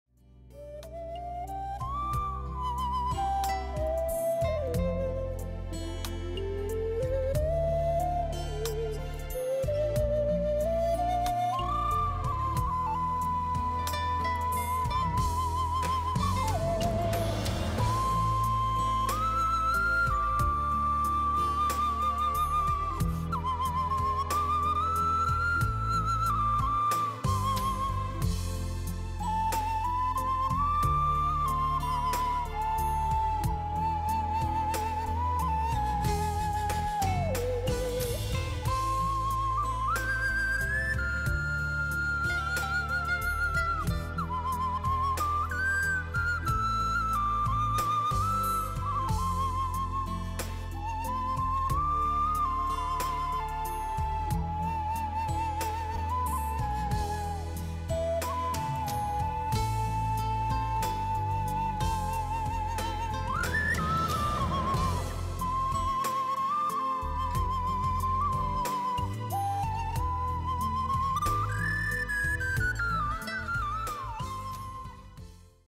Окарина - духовой музыкальный инструмент
Будьте_как_дома Окарина - духовой музыкальный инструмент, род свистковой флейты без выдувного отверстия. Название «окарина» применяется как ко всему семейству свистковых флейт, так и к конкретной разновидности, изобретённой в Италии и используемой в классической музыке. Народные окарины используются по всему миру — в Латинской Америке, Китае, Африке, восточной Европе и других местах.